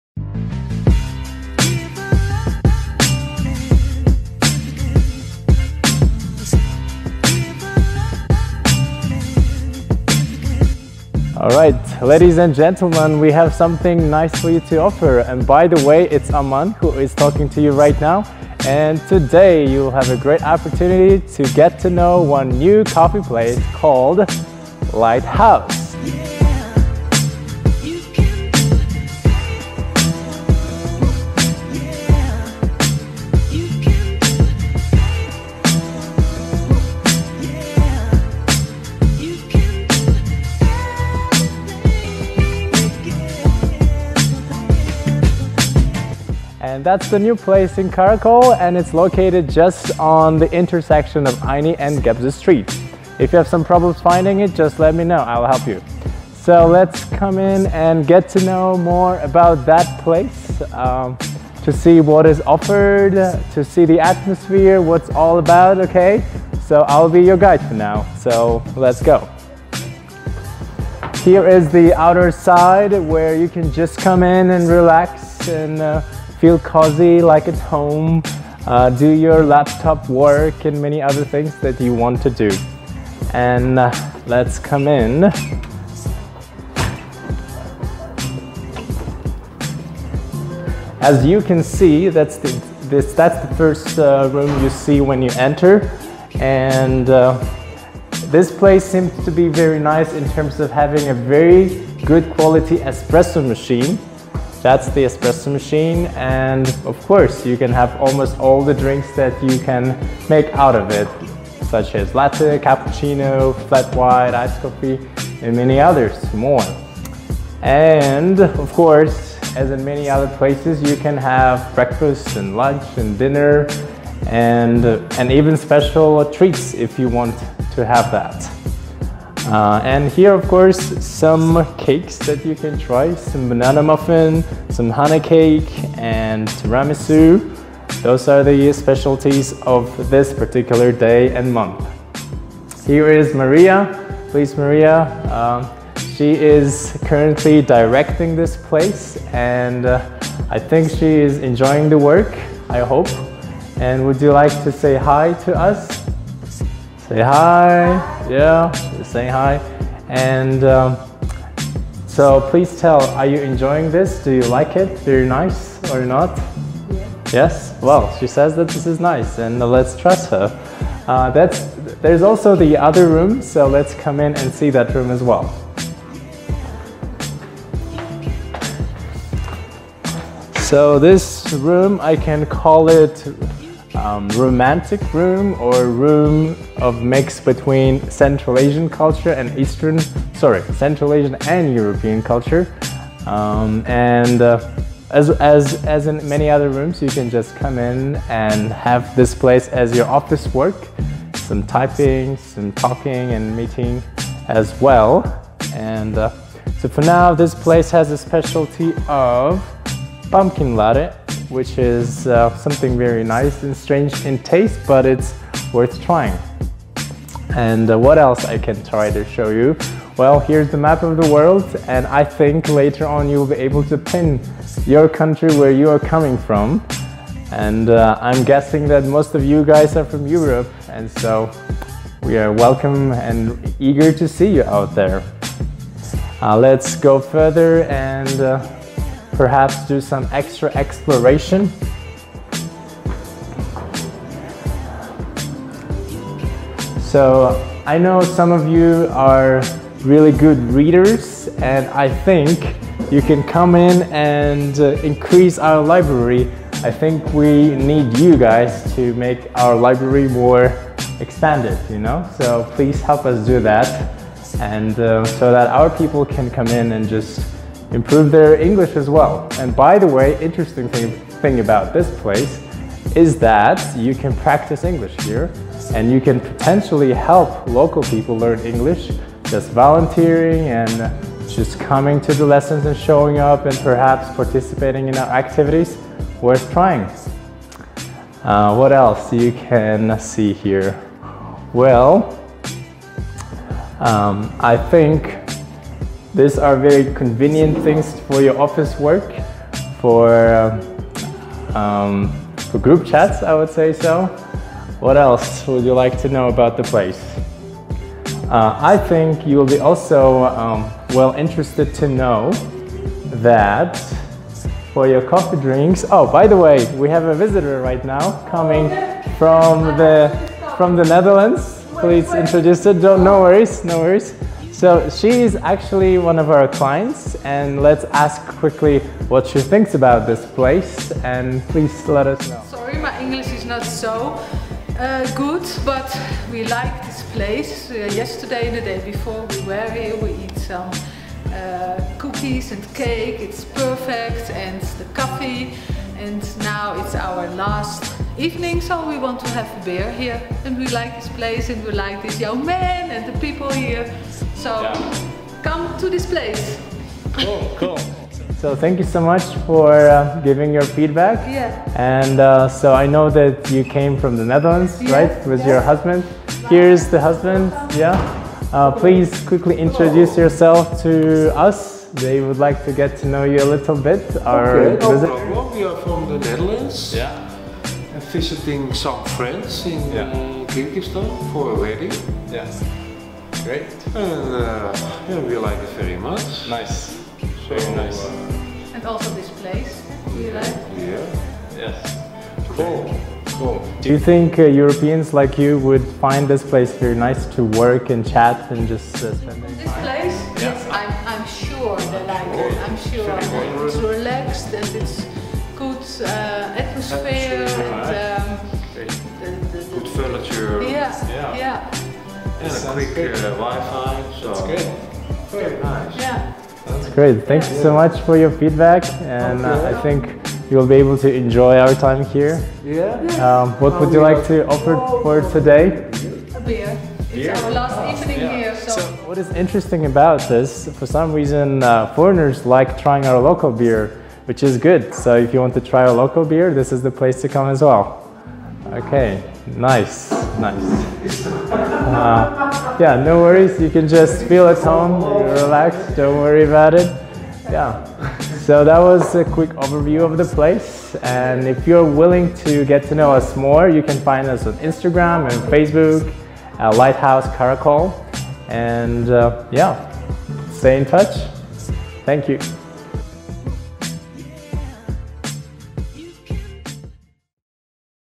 Visit Karakol TV: New Coffee Shop "Light House" in Karakol
Dear residents and guests of the city of Karakol!
We have good news for you! We are launching a new project Visit Karakol TV, which will cover the best tourist places, create useful video materials for tourists, cover festivals and events, what to see, what to eat, and also interesting life in Karakol.
We present to you the 1 series, where we told about the new coffee shop "Lighthouse" in Karakol city.